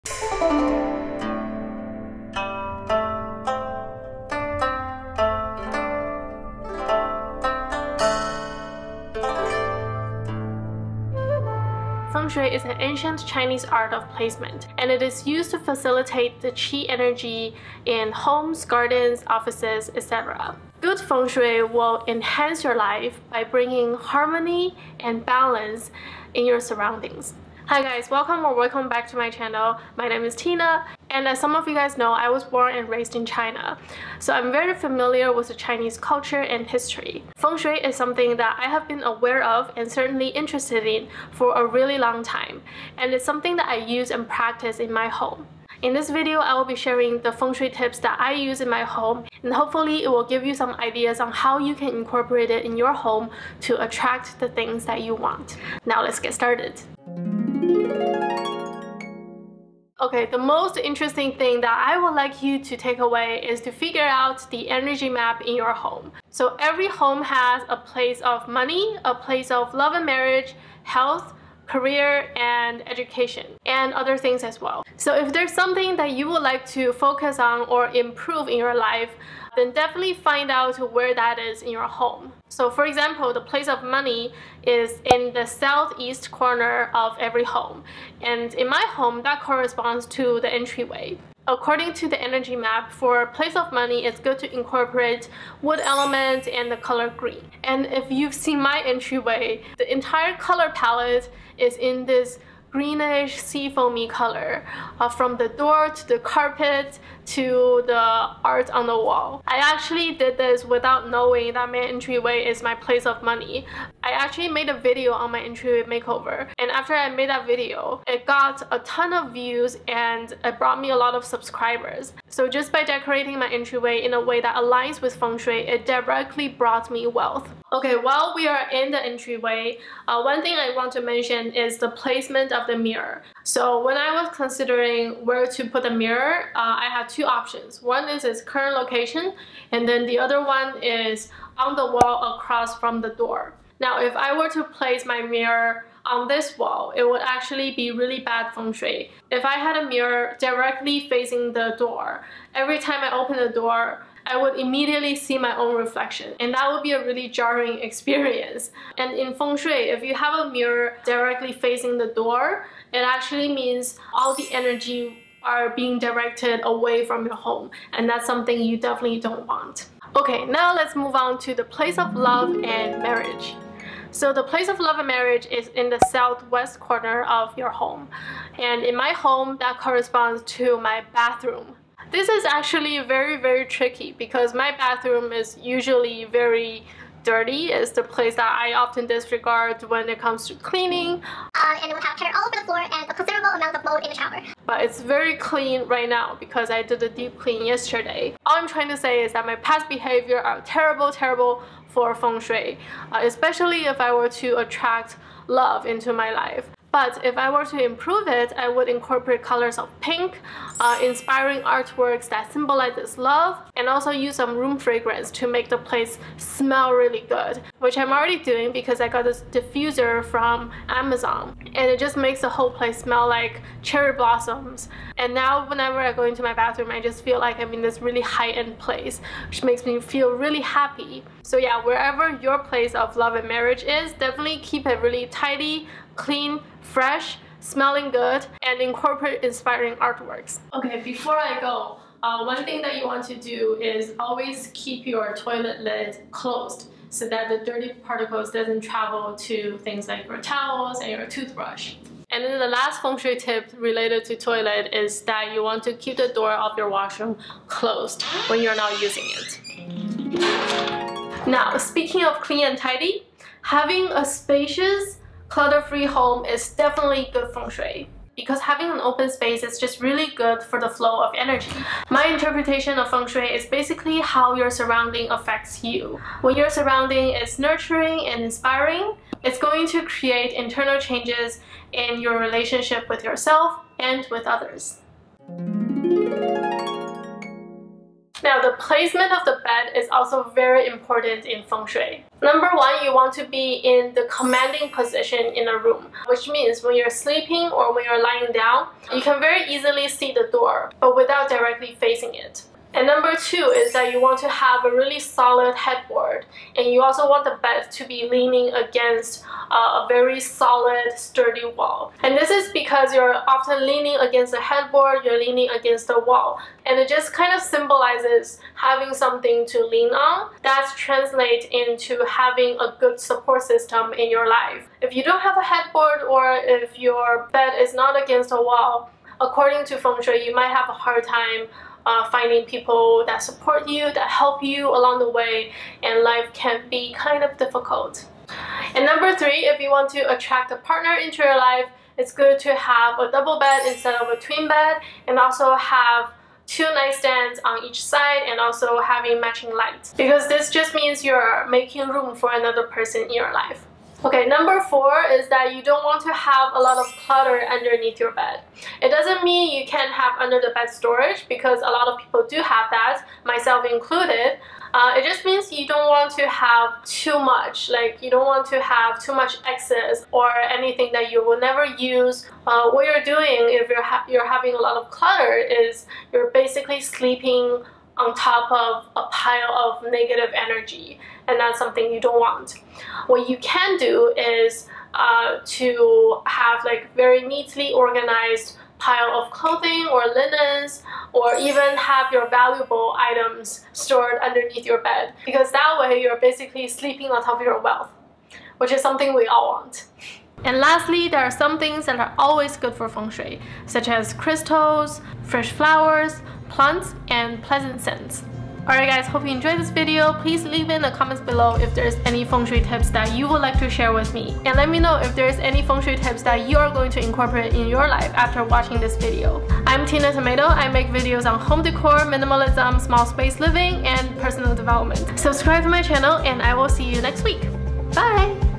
Feng Shui Tips in a Small Apartment – For More Love & Money 小户型风水
▶︎ Products Mentioned
Scent Diffuser

Feng shui is an ancient Chinese art of placement.  It is used to facilitate the flow of Ch’i in the home, garden, office, etc.  Good feng shui will enhance your life by bringing harmony and balance.
I’m Tina and as some of you know I was born and raised in China up to the age of 12. So I’m very familiar with Chinese culture and history. I’ve also been very aware and interested in Feng Shui and I try to practice it in my home to the best of my knowledge. Today, in this video I’ll be sharing the Feng Shui tips that I know and use in my home, and hopefully, it will give you some ideas on what you can incorporate your home so that you can attract the things that you want.